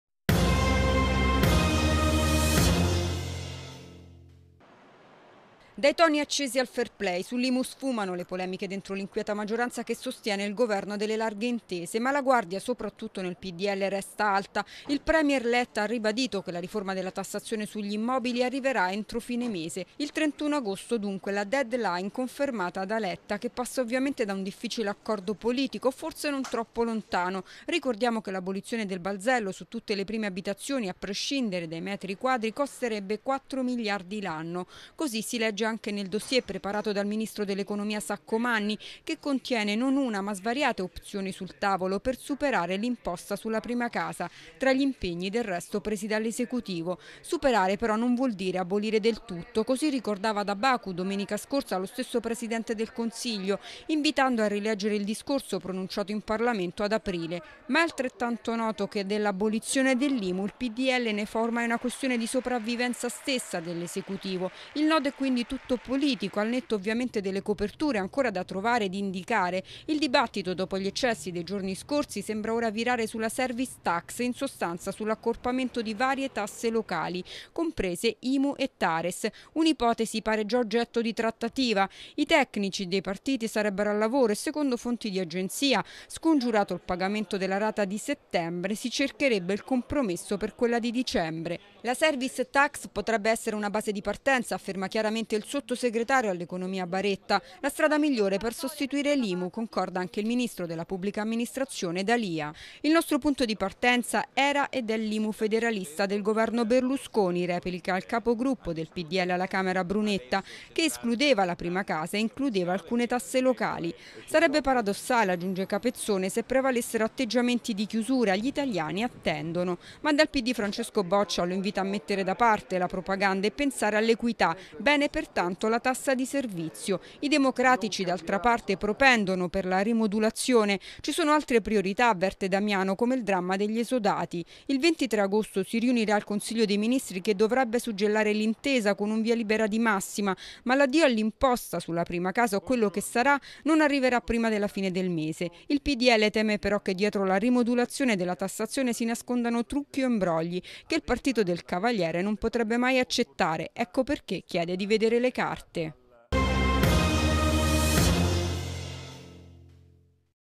Imu, al suo posto si pensa alla service tax
Dopo che il premier è tornato ieri a ribadire che sulla tassa sulla casa arriverà una decisione definitiva entro fine agosto, si ragiona su una tassa che comprenda più tributi compresa l'Imu. Servizio di Felicia Giudice